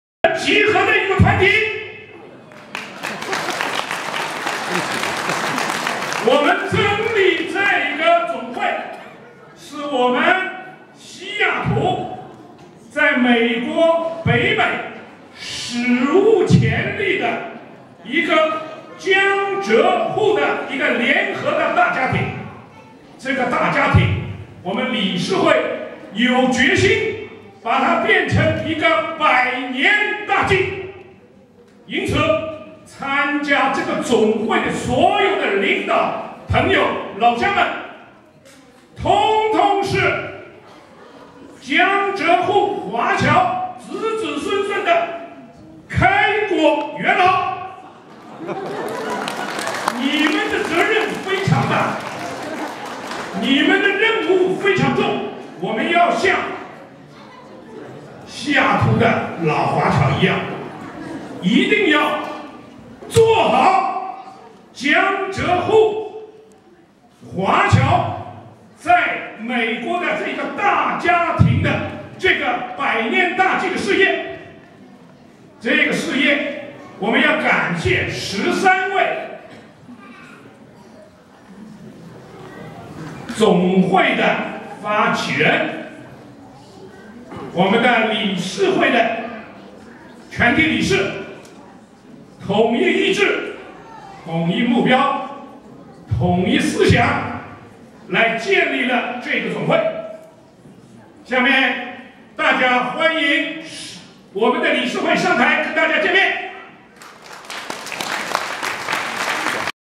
西雅图江浙沪同乡总会成立大会 --- 理事长李越致辞
Eastside Baha'i Center，Bellevue，2015年12月5日
西雅图中文电台电台报道